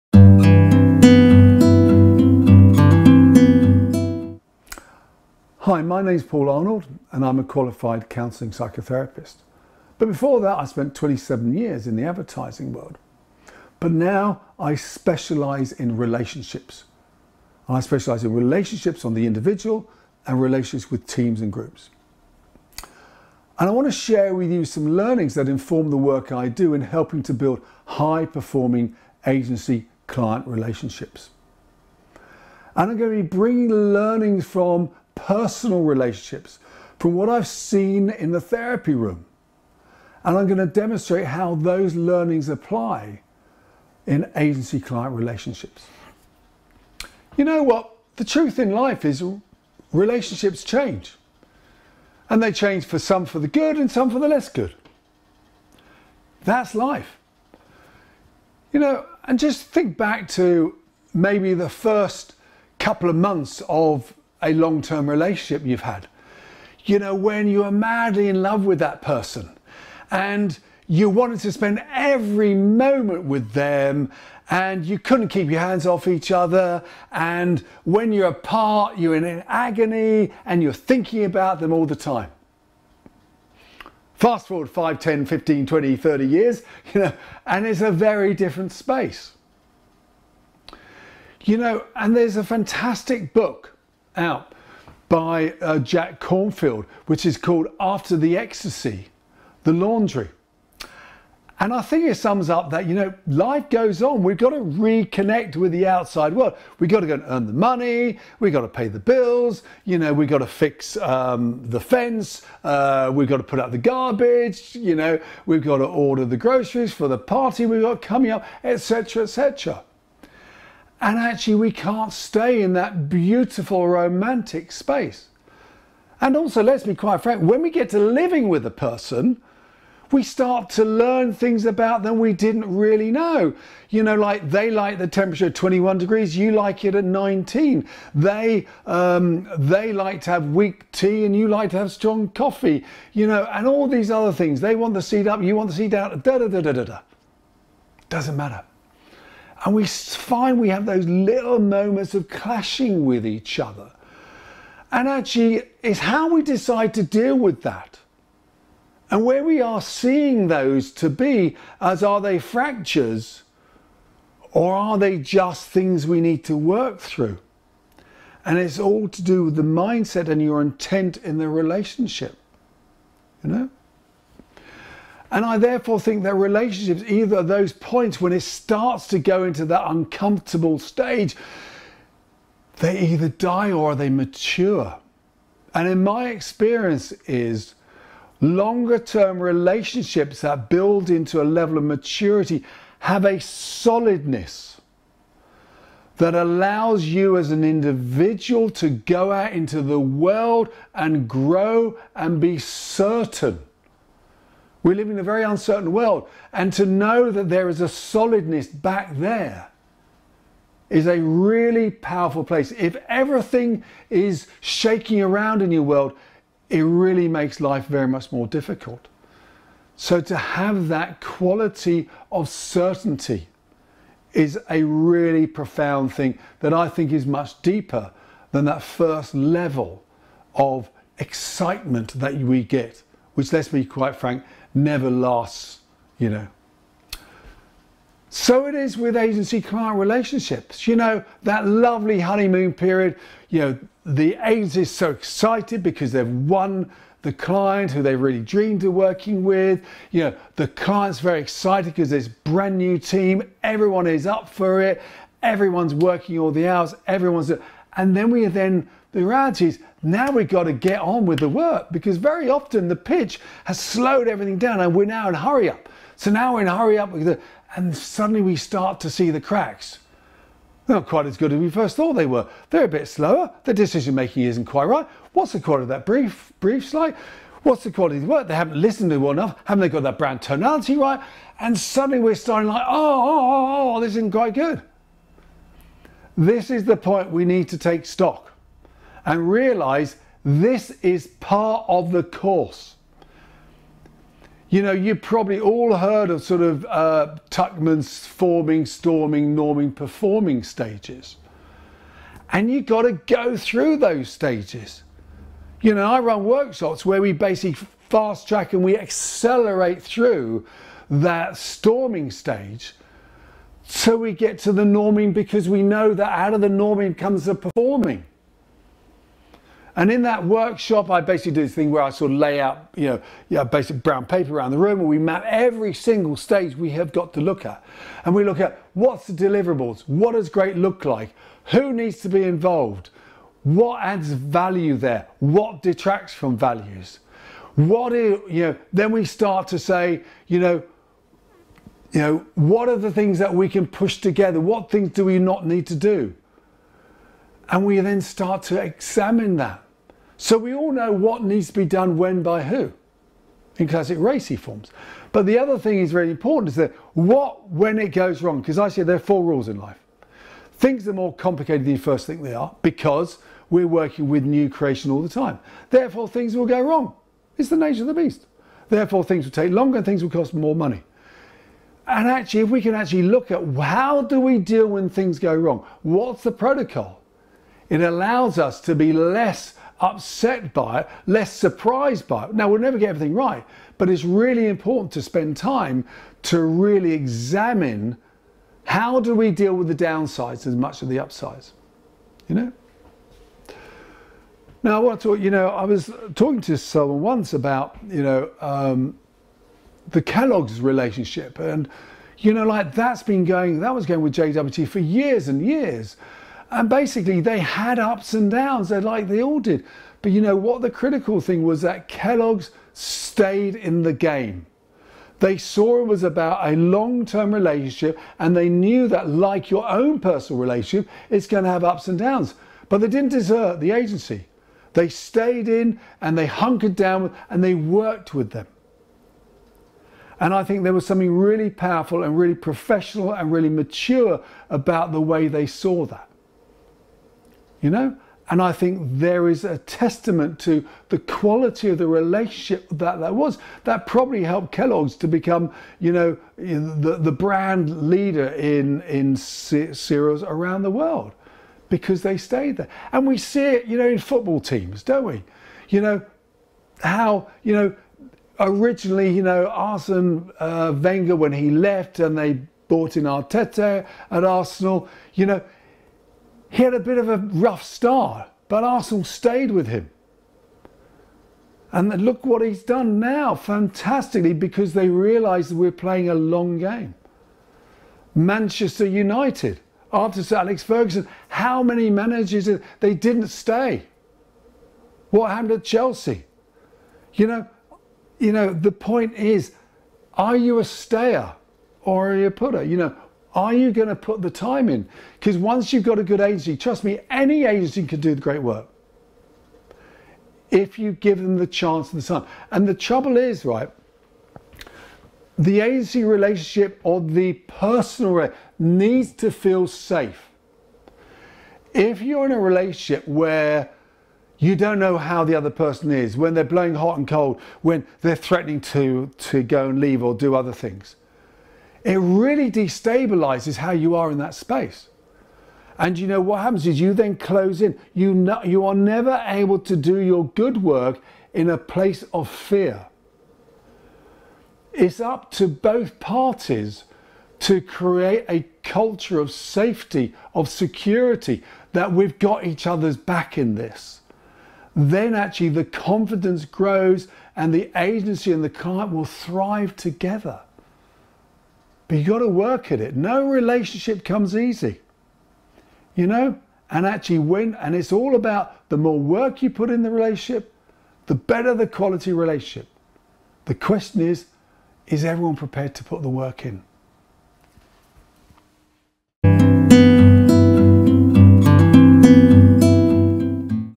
Client Agency Relationships 10 Relationships evolve
In this video I share learnings from my experience as a Psychotherapist (and ex Ad-man) to help develop high performing Client Agency teams